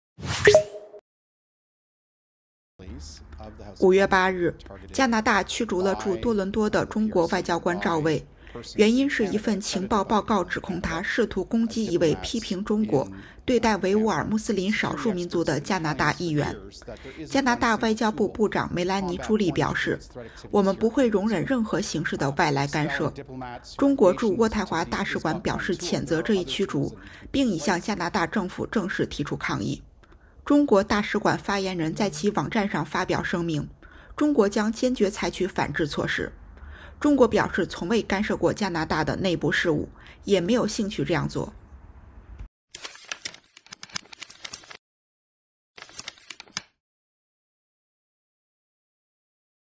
加拿大宣布驱逐中国外交官赵巍 中国：正式抗议
5月8日，加拿大驱逐了驻多伦多的中国外交官赵巍，原因是一份情报报告指控他试图攻击一位批评中国对待维吾尔穆斯林少数民族的加拿大议员。

加拿大外交部部长梅兰妮·朱莉表示：“我们不会容忍任何形式的外来干涉。”

中国驻渥太华大使馆表示谴责这一驱逐，并已向加拿大政府正式提出抗议。

中国大使馆发言人在其网站上发表声明：“中国将坚决采取反制措施。”

中国表示从未干涉过加拿大的内部事务，也没有兴趣这样做。